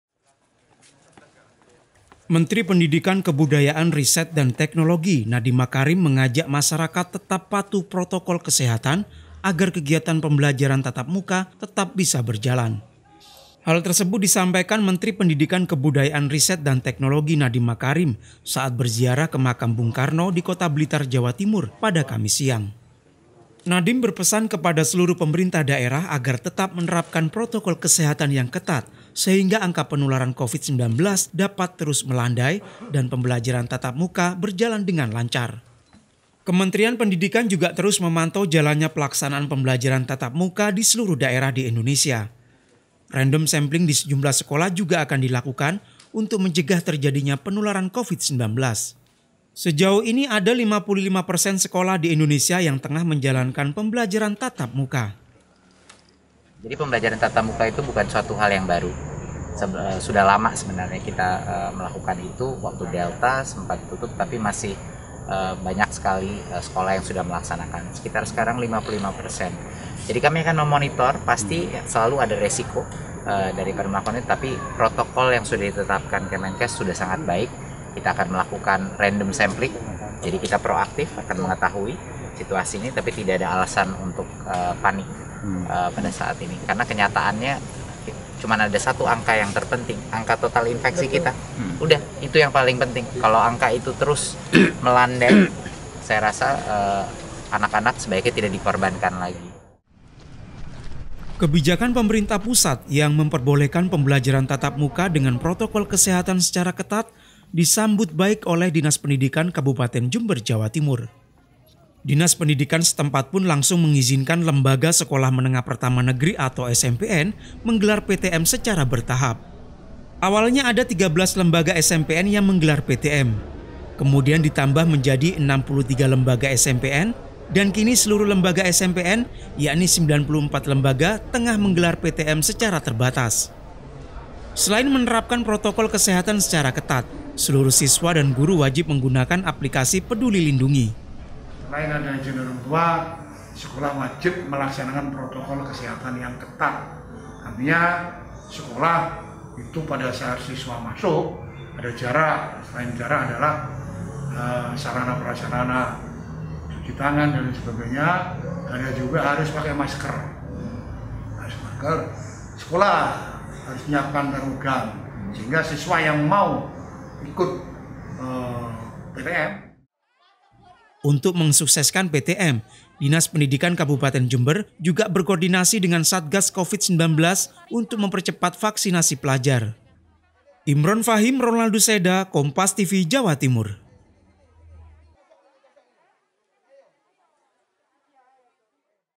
Kasus Covid-19 Menurun, Nadiem: Patuhi Protokol Kesehatan Agar PTM Tetap Bisa Digelar
JAWA TIMUR, KOMPAS.TV - Menteri Pendidikan Kebudayaan Riset dan Teknologi, Nadiem Makarim mengajak masyarakat agar tetap patuh pada protokol kesehatan agar kegiatan pembelajaran tatap muka tetap bisa berjalan.

Hal tersebut disampaikan Mendikbud Ristek, Nadiem Makarim saat berziarah ke makam Bung Karno di kota Blitar Jawa Timur, pada Kamis (21/10/2021).

Nadiem Makarim berpesan kepada seluruh pemerintah daerah agar tetap menerapkan protokol kesehatan yang ketat, sehingga angka penularan Covid-19 dapat terus melandai dan pembelajaran tatap muka berjalan dengan lancar.

Kemendikbud Ristek terus memantau jalannya pelaksanaan pembelajaran tatap muka di seluruh daerah di Indonesia. Random sampling di sejumlah sekolah juga akan dilakukan untuk mencegah terjadinya penularan Covid-19. Nadiem menyebut saat ini sebanyak 55 persen sekolah di Indonesia telah menjalankan pembelajaran tatap muka.

Kebijakan pemerintah yang memperbolehkan pembelajaran tatap muka disambut baik oleh Dinas Pendidikan Kabupaten Jember Jawa Timur. Dinas Pendidikan setempat pun langsung mengizinkan sekolah menengah pertama negeri atau SMPN menggelar PTM secara bertahap.

Baca Juga 63 SMP di Jember Menggelar PTM Terbatas, Siswa dan Guru Wajib Lakukan Hal ini di

Awalnya 13 lembaga SMPN, kemudian ditambah menjadi 63 lembaga dan kini seluruh lembaga SMPN, yakni 94 lembaga tengah menggelar PTM secara terbatas.

Kepala Dinas Pendidikan Kabupaten Jember, Bambang Hariono mengatakan bahwa bahwa selain menerapkan protokol kesehatan secara ketat, seluruh siswa dan guru wajib menggunakan aplikasi peduli lindungi.

Untuk mensukseskan PTM, Dinas Pendidikan Kabupaten Jember juga berkoordinasi satgas Covid-19 untuk mempercepat vaksinasi pelajar.